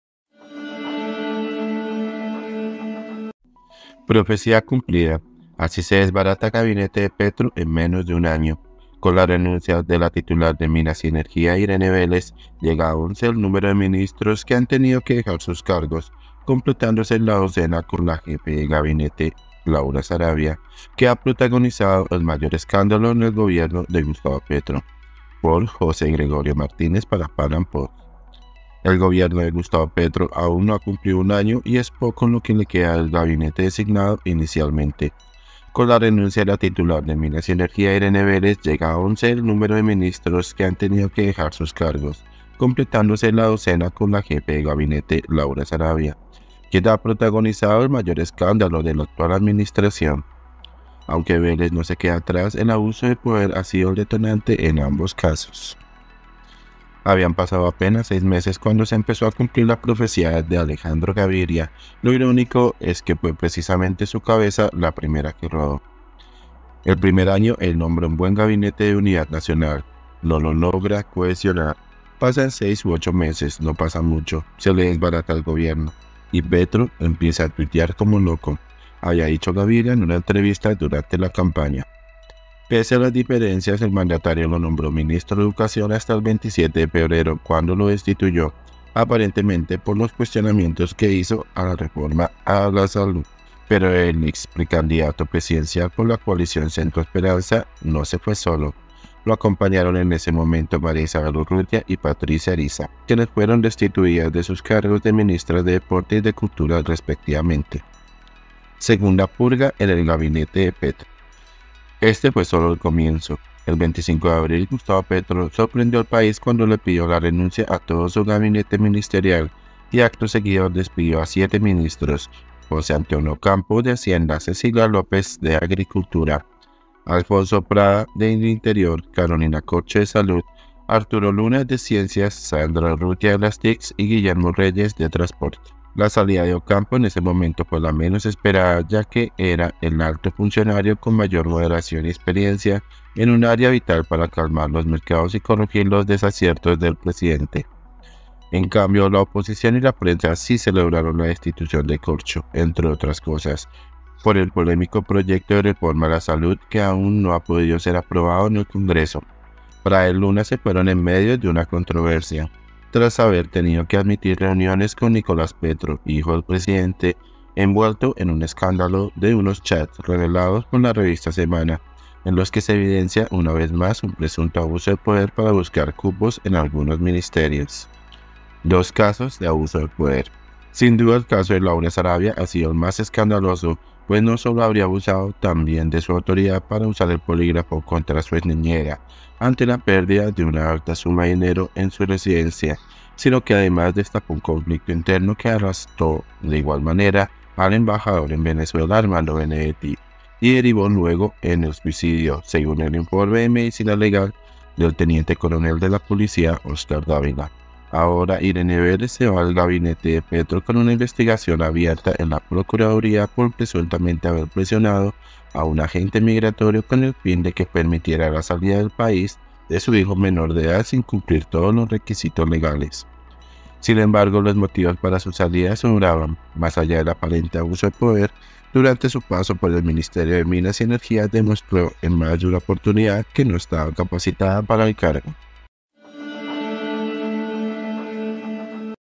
🔴 PROFECIA CUMPLIDA: así “se desbarata” gabinete de Petro en menos de un año
Con la renuncia de la titular de Minas y Energía, Irene Vélez, llega a 11 el número de ministros que han tenido que dejar sus cargos, completándose la docena con la jefa de gabinete, Laura Sarabia, que ha protagonizado el mayor escándalo en el Gobierno de Gustavo Petro.
 por José Gregorio Martínez PANAM POST